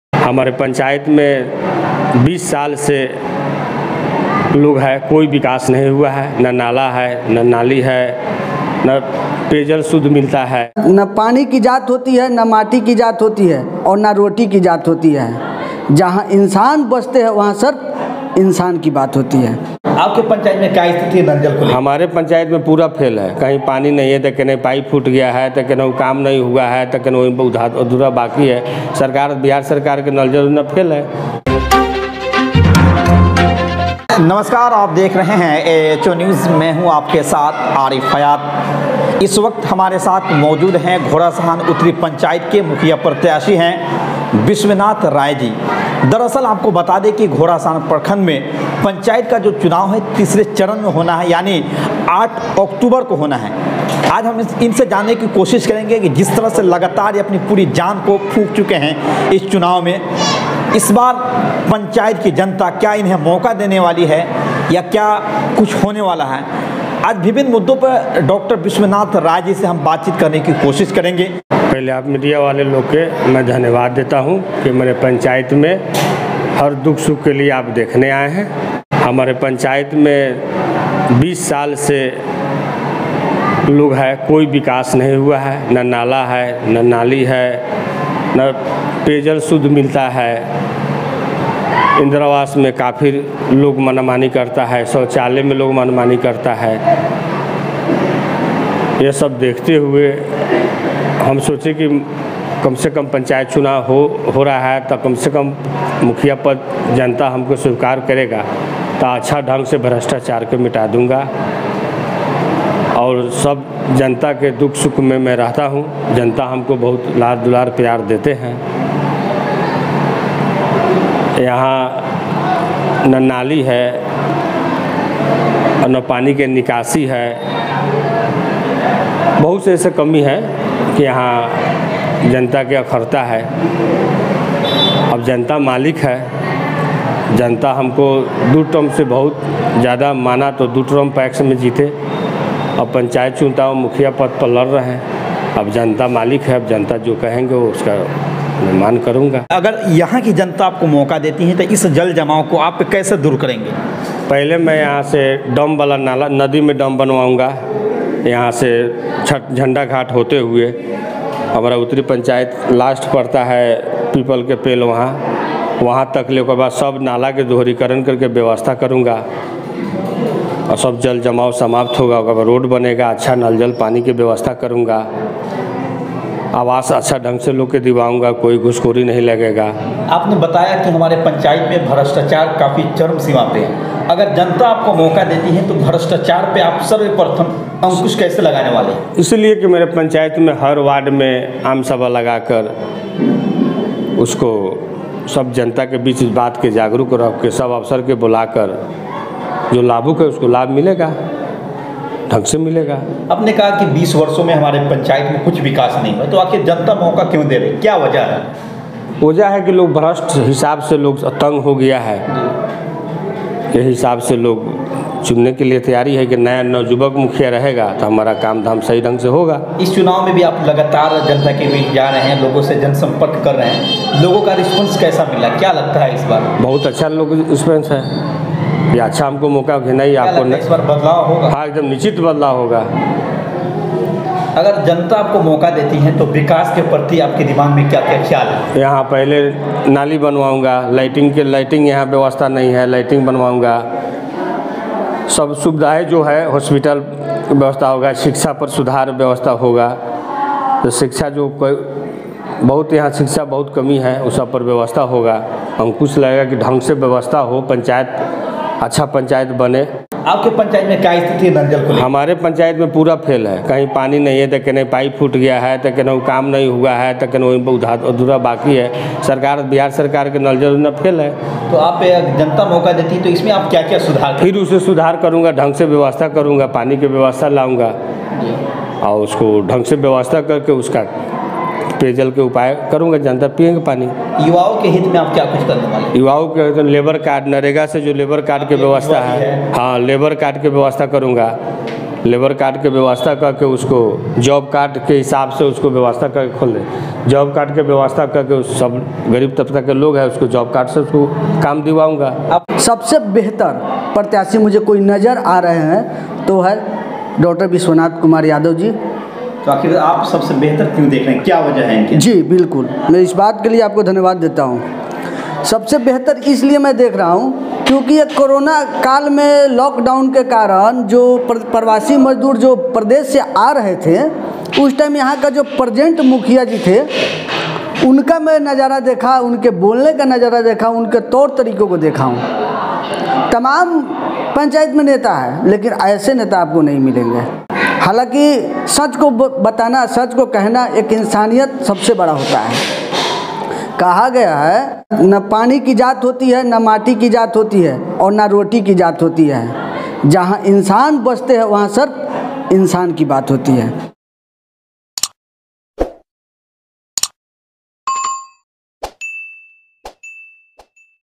घोड़ासहन उत्तरी पंचायत के मुखिया प्रत्याशी विश्वनाथ राय ने चुनाव से पहले किया जीत का दावा
घोड़ासहन उत्तरी पंचायत के मुखिया प्रत्याशी विश्वनाथ राय ने चुनाव से पहले किया जीत का दावा देखिए धमाकेदार इंटरव्यू